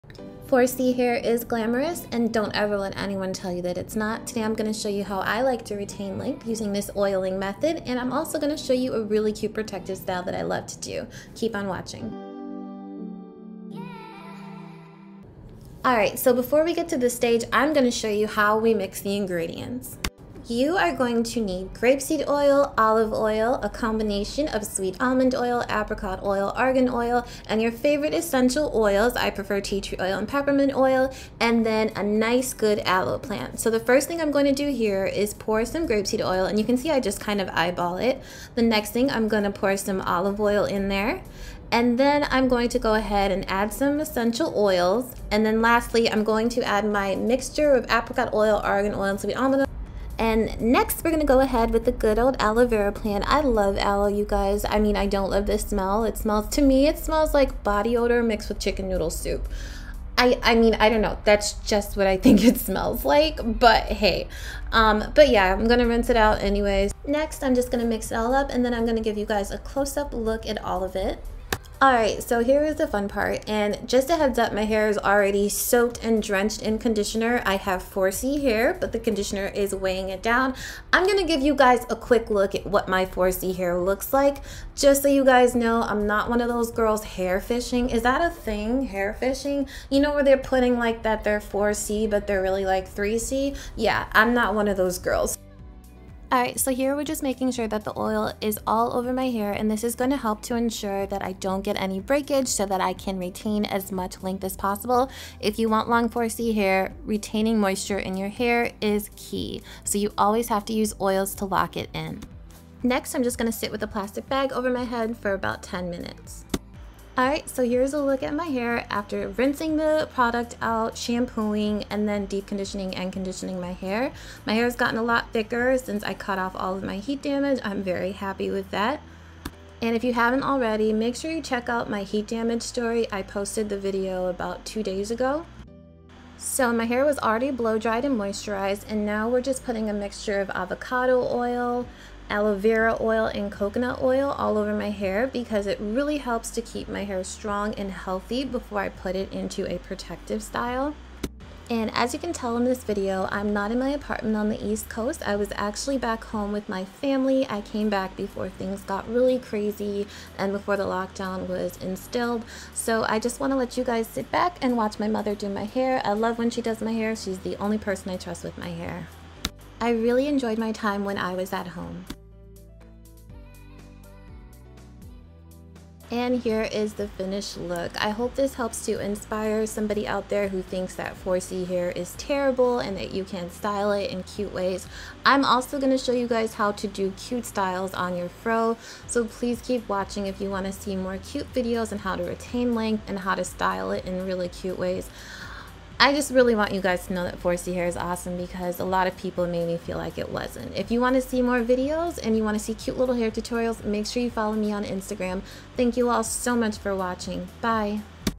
Do This Aloe Vera Treatment For EXTREME 4C Hair Growth FAST
Hi Loves! I wanted to show you an oiling method I use, to retain length and stimulate hair growth. It's really easy to do, I hope it helps.

I used:

Grapeseed Oil
Olive Oil
Apricot Oil, Sweet Almond Oil, Argan Oil
Aloe Vera Oil, Coconut Oil, Avocado Oil
Aloe Vera Plant/Gel